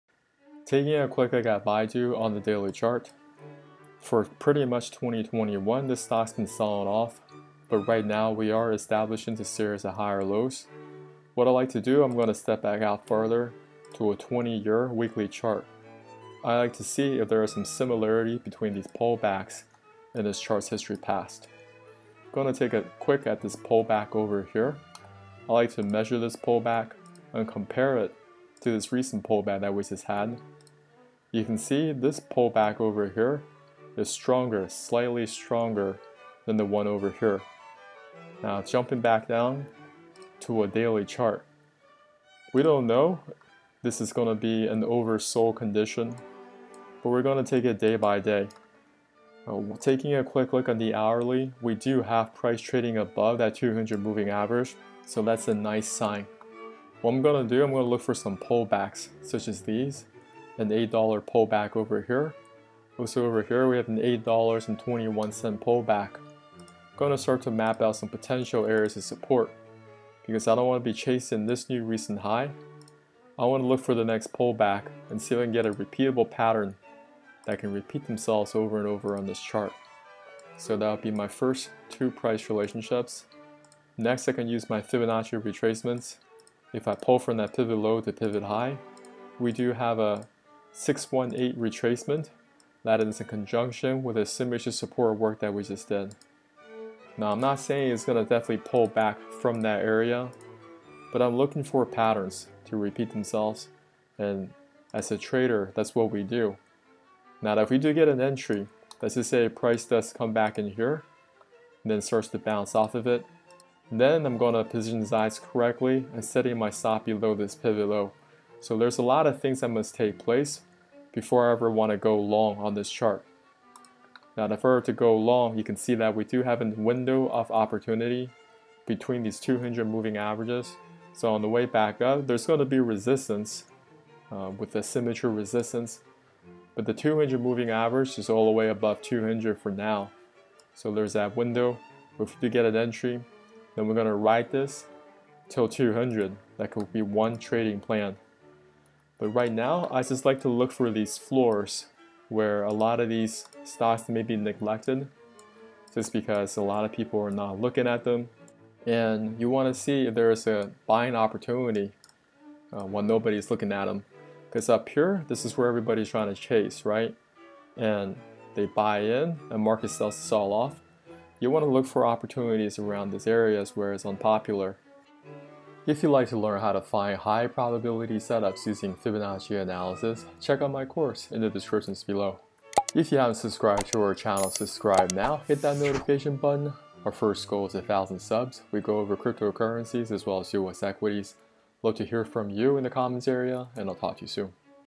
BAIDU ($BIDU) - Possible Bullish 4th Qtr? Technical Fibonacci Analysis. Study Price Patterns.
𝗜'𝗺 𝗶𝗻𝘃𝗶𝘁𝗶𝗻𝗴 𝘆𝗼𝘂 𝘁𝗼 𝘀𝘁𝗮𝗿𝘁 𝗶𝗻𝘃𝗲𝘀𝘁𝗶𝗻𝗴 𝗶𝗻 𝗰𝗿𝘆𝗽𝘁𝗼 𝘄𝗶𝘁𝗵 𝗩𝗼𝘆𝗮𝗴𝗲𝗿 𝗔𝗽𝗽.

💡 BitCoin: 3JQnmjFCMLAkw1yQEA4oVSXk7k4LsiEiWK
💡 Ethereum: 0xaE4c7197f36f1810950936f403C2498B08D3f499

- 𝗖𝗼𝗺𝗽𝗼𝘀𝘂𝗿𝗲
- 𝗖𝗹𝗮𝗿𝗶𝘁𝘆
- 𝗖𝗼𝗺𝗺𝗶𝘁𝗺𝗲𝗻𝘁